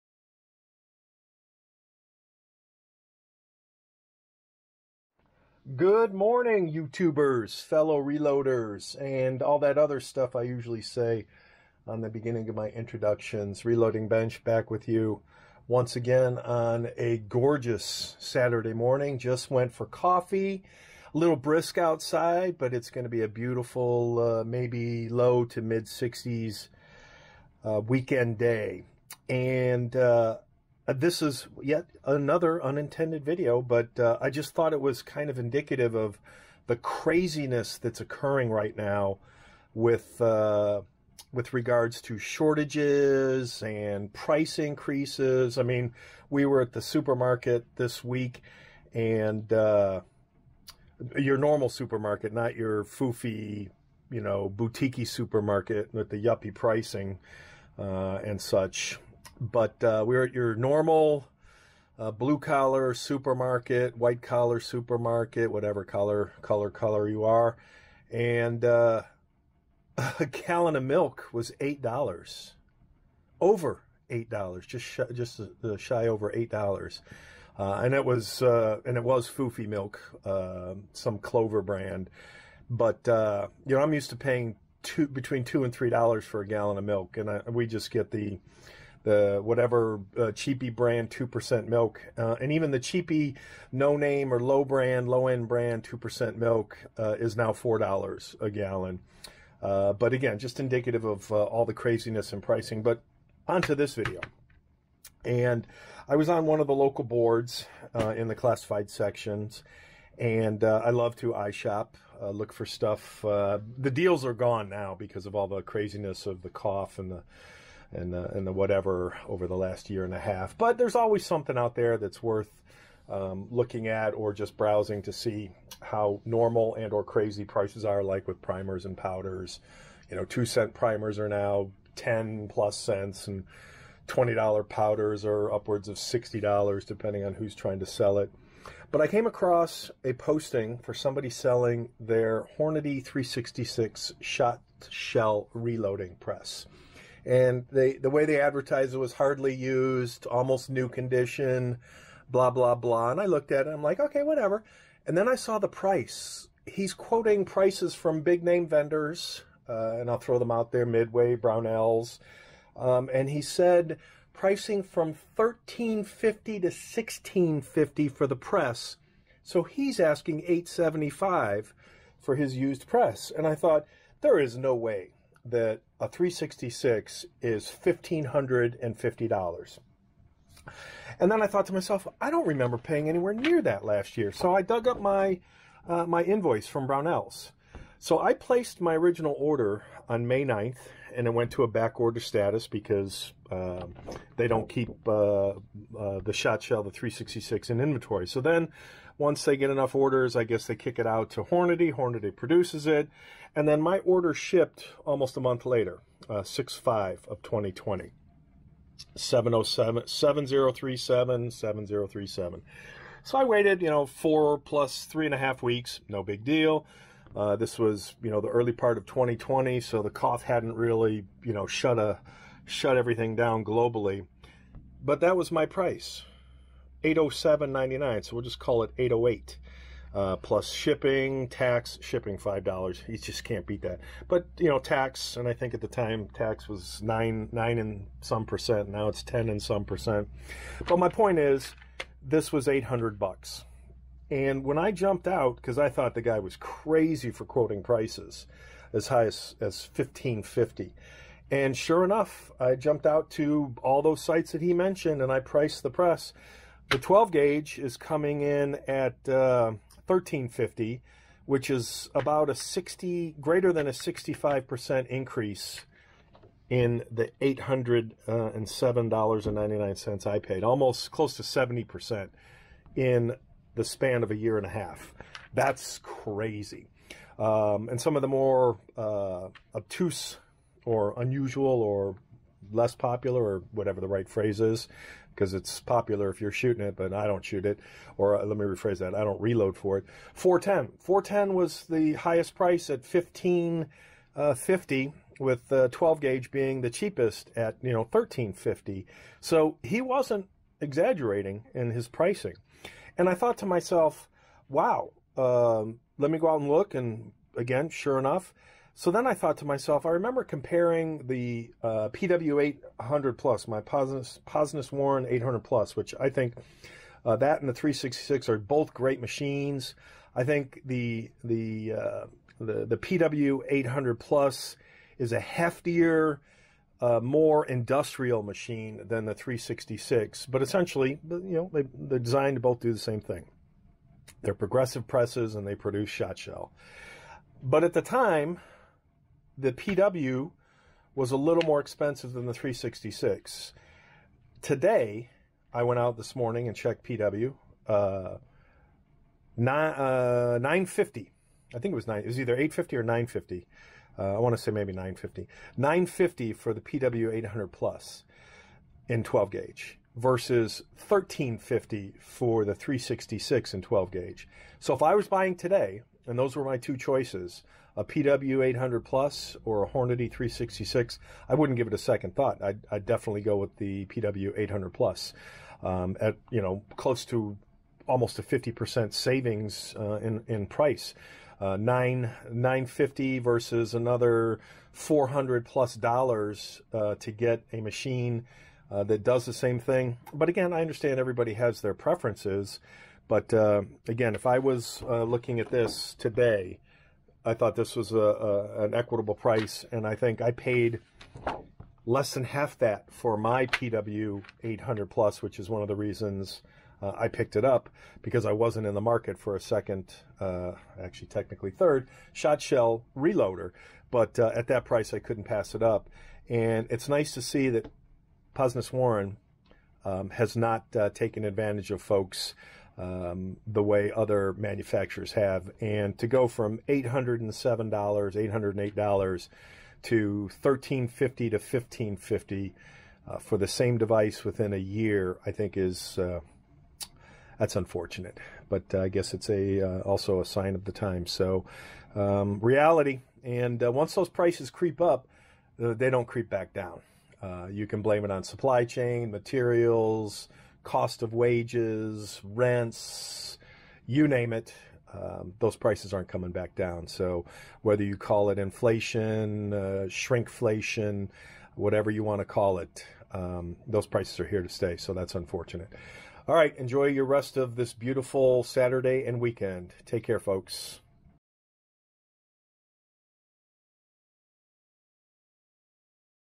WOW! - Crazy Price Increases and or Long Lead Times for Shotshell Reloading Progressive Presses
Another unintended video resulting from my search of local classified for reloading related gear.  Someone was selling a used Hornady 366 and they were referencing current pricing for the same 366 @ $1,350-1,550.  I thought they were crazy until I searched the sites/vendors he referenced and sure enough, he was not exagerating.

I looked up my old invoice from earlier in 2020 and I paid just over $800 for my 366 brand new.  Exact same model is now selling for almost 70% more at $1,350.  Also checked the current listed price of a PW 800 Plus and it's relatively unchanged at $950.  If I was buying today, the choice would be easy, a PW.  $1,350 versus $950, no brainer at least to me.

Unfortunately, due to all the global issues (pick any one), the lead time on the PW is a YEAR or more.  Not sure about the 366 as they are listed as "special order" but lead times are listed as comparable to what I experienced in early 2020.